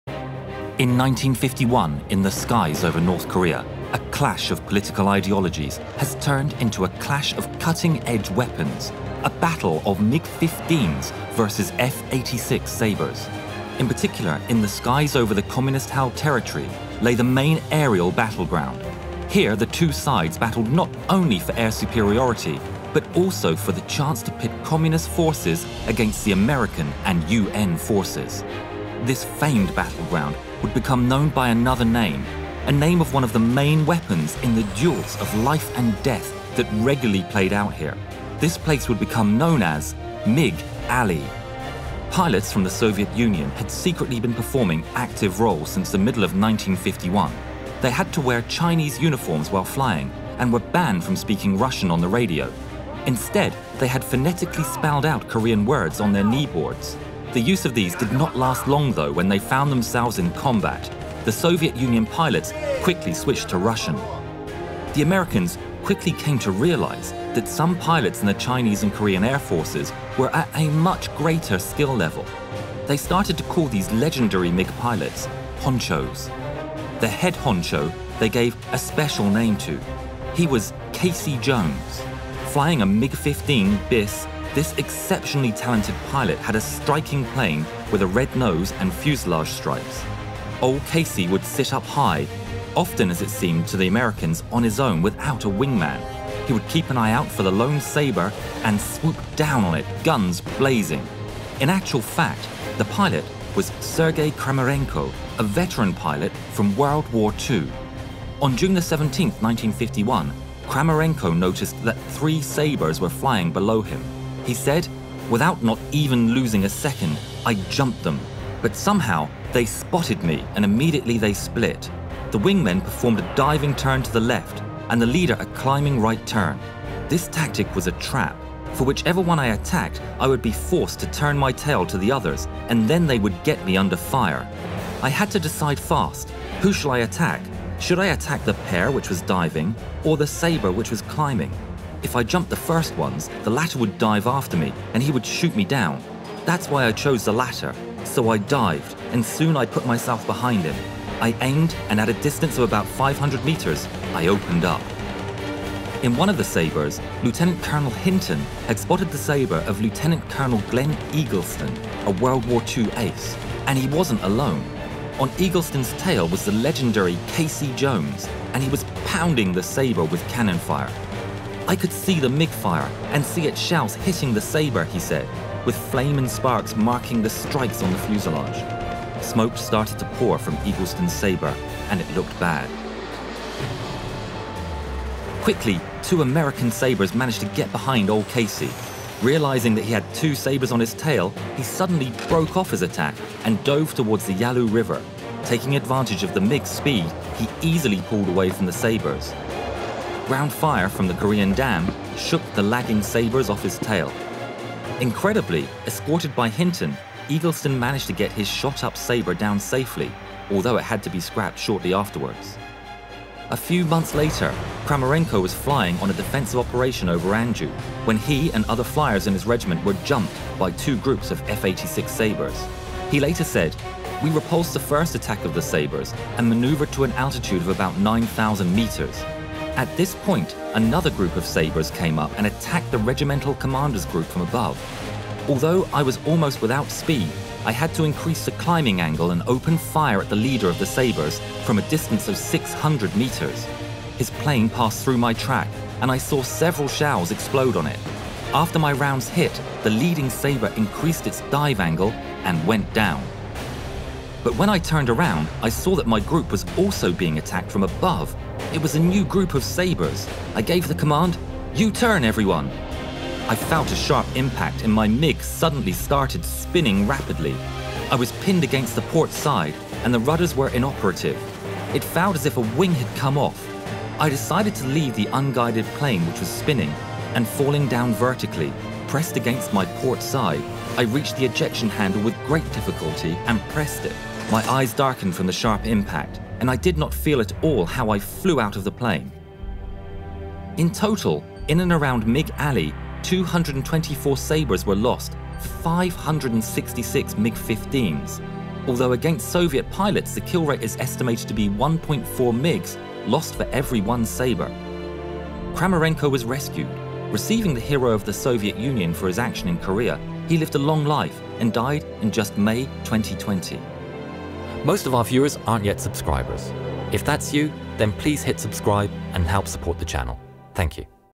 When Secret Soviet MiG Aces Fought US Sabres
In the skies over Korea at the height of the cold war, Soviet fighter aces secretly fought against the UN forces. Northwest  Korea over the Yalu river, also known as Mig Alley is where most of these engagements occurred.  The US pilots named these talented aviators "Honchos".  The Head Honcho that patrolled over Mig Alley, was nicknamed "Casey Jones" or "Ol' Casey".  Flying a MIG 15 bis with a special paint job, Casey Jones was none other than  Soviet WW2 pilot Sergei Kramarenko.

Thanks to Vika for translating the information from the Russian sources. In particular Sergei Kramarenko's memoire, "Against Messerschmitts and Sabre Jets. In the Sky of Two Wars" by Sergei Kramarenko

We also took references from the excellent book F-86 Sabre vs MiG-15 : Korea 1950-53 by  Douglas C. Dildy  and Warren Thompson

Yarnhub uses the Unreal® Engine. Unreal® is a trademark or registered trademark of Epic Games, Inc. in the United States of America and elsewhere.